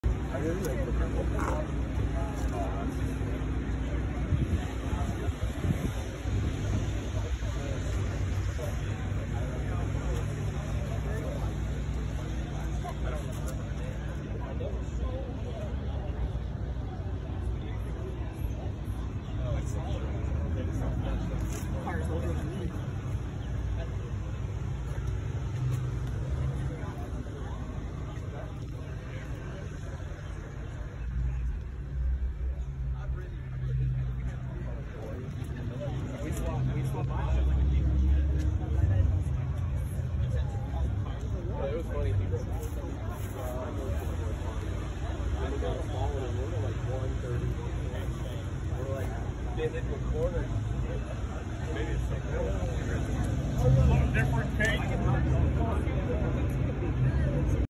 Yellow 76 Corvette Spotted in Gainesville
This beautiful 1976 Corvette Stingray was at Gainesville Cars and Coffee, Sunday, October 9, 2022.

I met the owners, Doug and Jane, who also live in Ocala, so I hope they’ll join us at a few local events.

And I’ll try to do a more in-depth video about the Vette one of these days as it’s been modified and updated quite a bit.